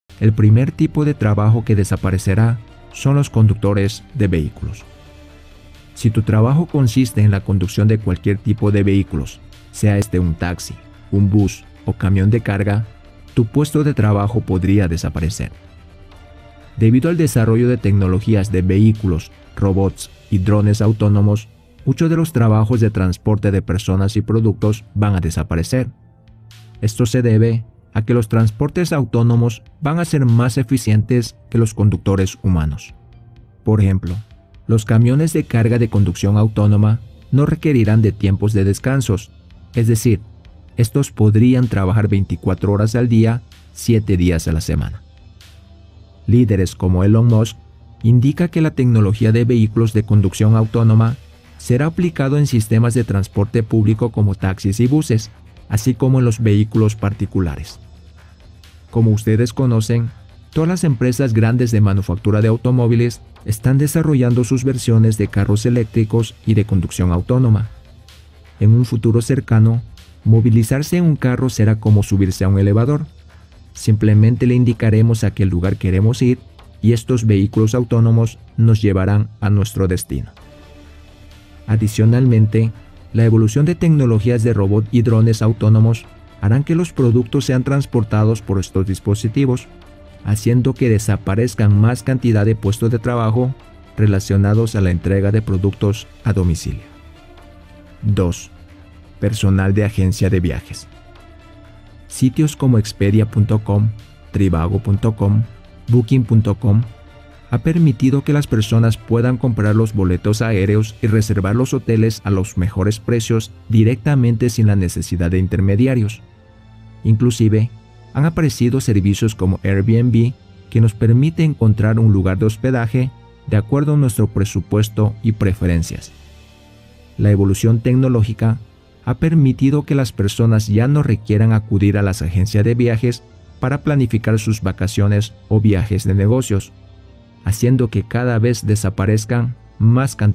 Trabajos que desaparecerán en 20 años
Los empleos que más riesgo corren son, los de contables y auditores, así como los relacionados con actividades que trabajan de cara al público, como agentes de viajes, cajeros, secretarios, recepcionistas, agentes de seguros, teleoperadores que ofrecen servicios de atención al cliente y empleados de banca. También los que tienen que ver con el transporte, como maquinistas de tren y metro, conductores y taxistas, y mecánicos y revisores de aviones. En el otro extremo, los trabajos con más salidas para la próxima década serían los relacionados con las Ciencias de la Salud y las Tecnologías de la Información y la Comunicación (TIC).